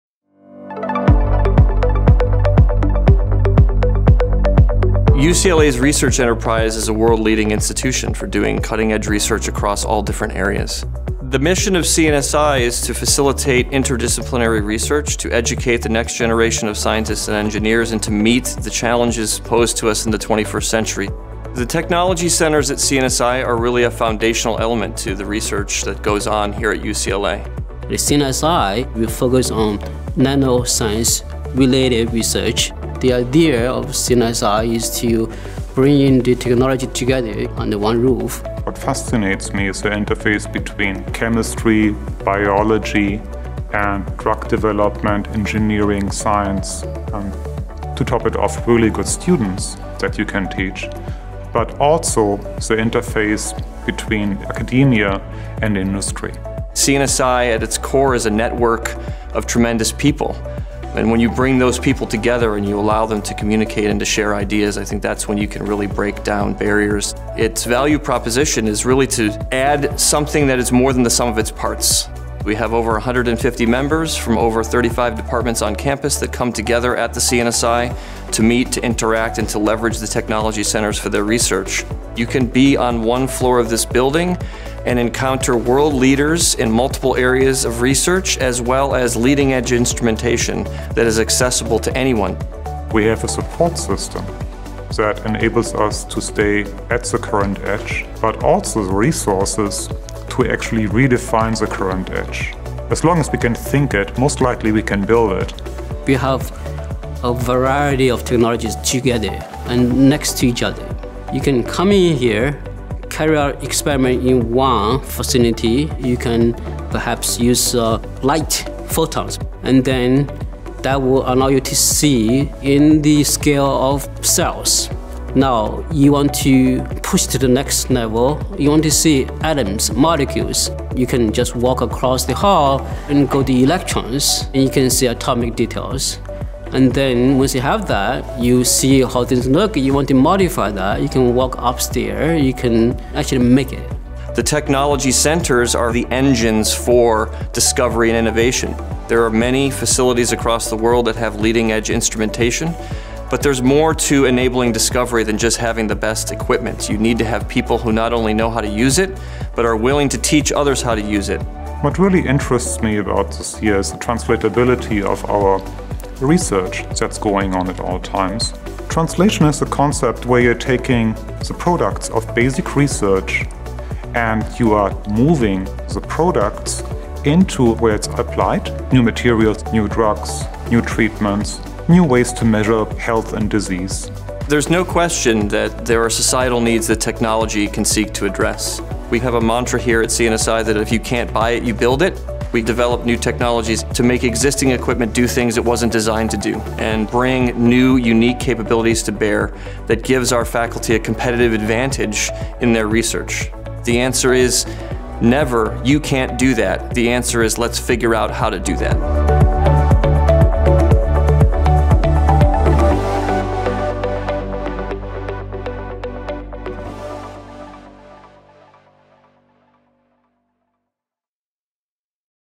CNSI Technology Centers - Meeting the challenges posed to us in the 21st century
The CNSI Technology Centers provide unrestricted access to fully integrated R&D infrastructure, professional support and collaborative research opportunities in the fabrication, characterization and screening of nanomaterials, systems, and devices. Directed by a group of leading scientists, the Centers develop technologies that promote discovery through direct grant support, user proposals and industrial partnerships. Access to these facilities is available 24 hours a day, 365 days a year for students, postdocs, staff, faculty and industrial scientists.